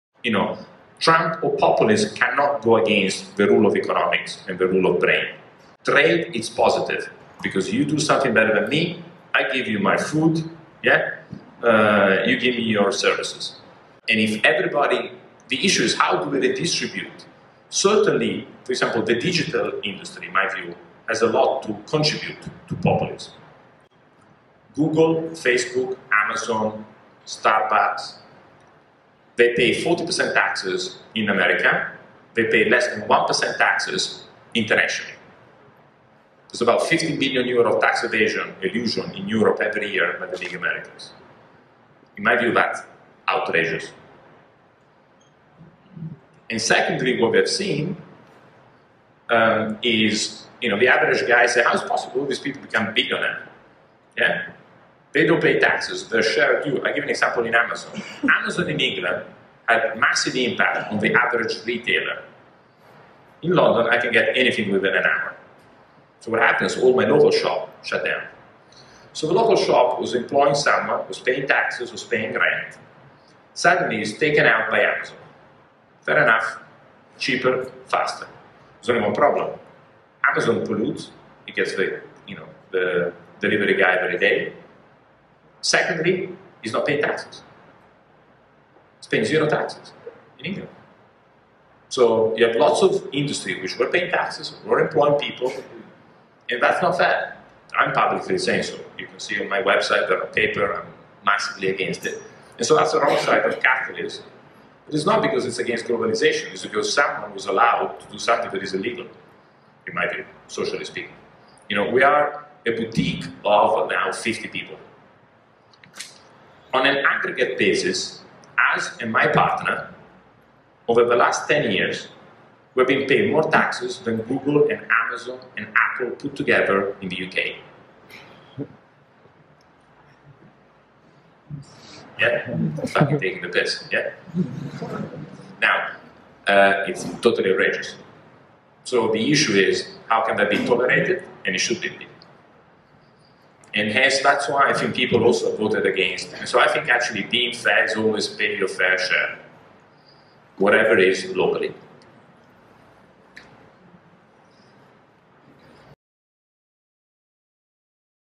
Davide Serra on digital industry, taxes and populism
17/11/2016 @ SanteriaSocialClub, Milan

Hosted by B.lab, Unilab Svoltastudenti, Bocconi Students Investment Club, European Generation & BS Private Equity Club.